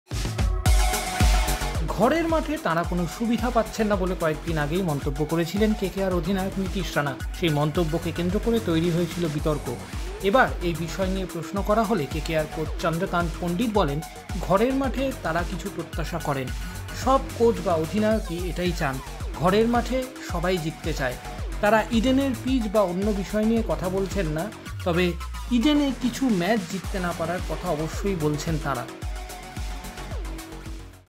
'ঘরের মাঠে তো সবাই জিততে চায়', ইডেনের পিচ বিতর্কে এবার মন্তব্য কেকেআর কোচের
ঘরের মাঠে আমরা কিছু প্রত্যাশা করি। সব কোচ বা অধিনায়কই তাই চান। ঘরের মাঠে তো সবাই জিততে চায়।